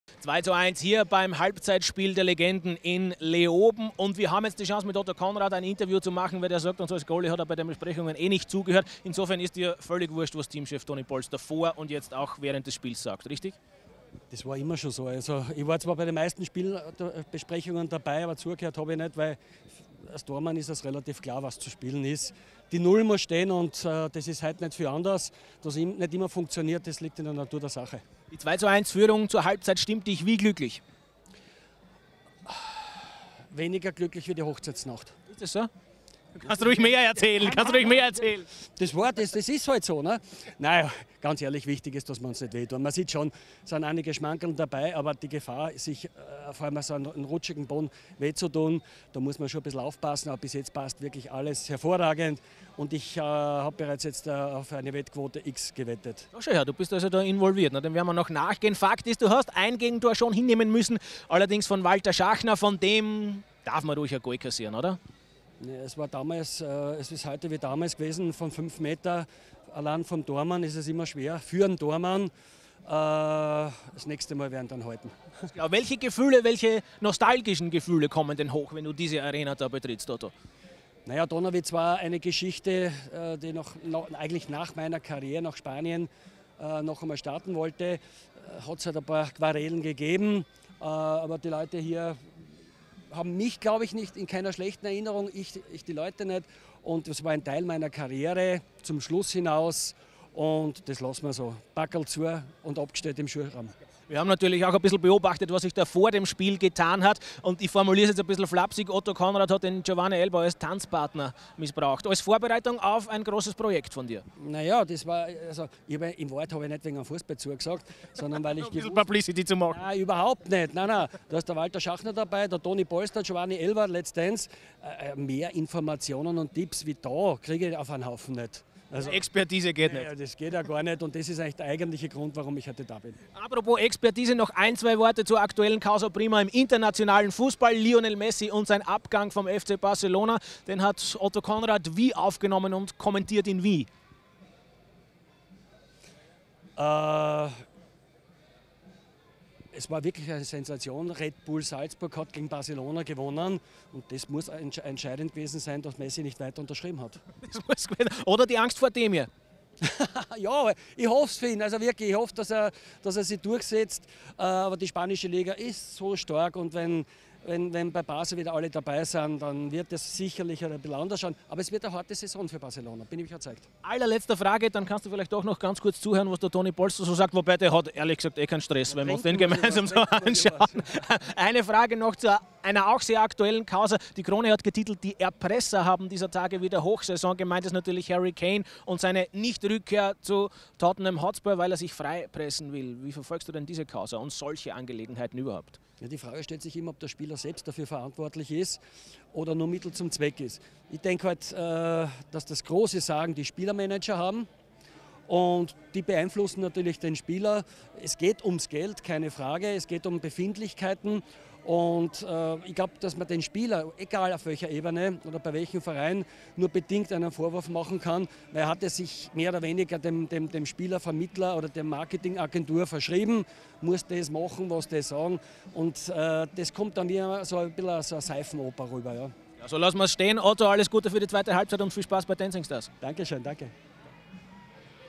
Konrad: Pleite gegen Salzburg für Messi zu viel? | krone.tv Sport-Talk
Auch Otto Konrad gab sich beim Legenden-Spiel in Leoben die Ehre. Der Kult-Goalie und künftige „Dancing Star“ sprach zur Halbzeit mit Michael Fally über seine Leistung, das Tor von Walter Schachner, seine Tanz-Ambitionen - und er scherzte auch über Lionel Messis Abgang von Barcelona. Konrad schmunzelnd: „Barca hat gegen Salzburg verloren - das war für Messi offenbar ausschlaggebend, dass er nicht mehr unterschrieben hat.“